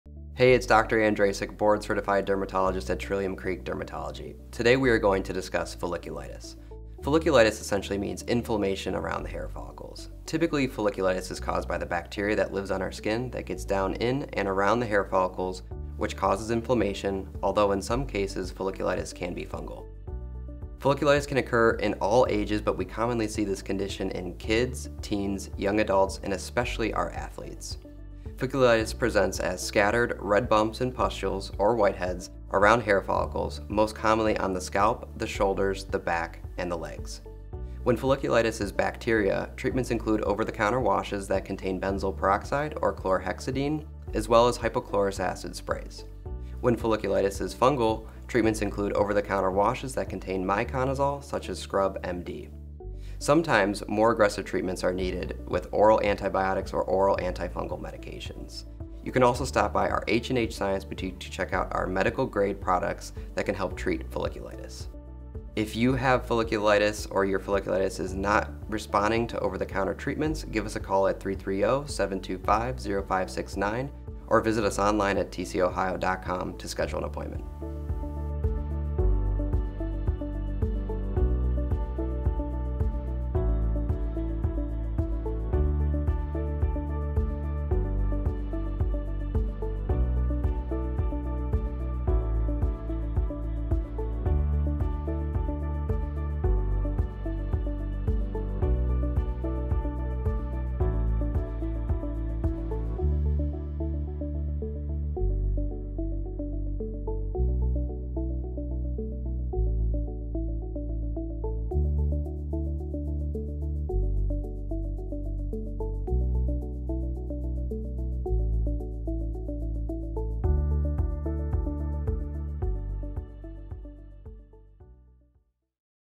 What is folliculitis?
Dr. Wyatt Andrasik, Dermatologist at Trillium Creek Dermatology, discusses folliculitis. This inflammation of the hair follicle can be bacterial or fungal, so it is best to see your skin expert for the correct treatment.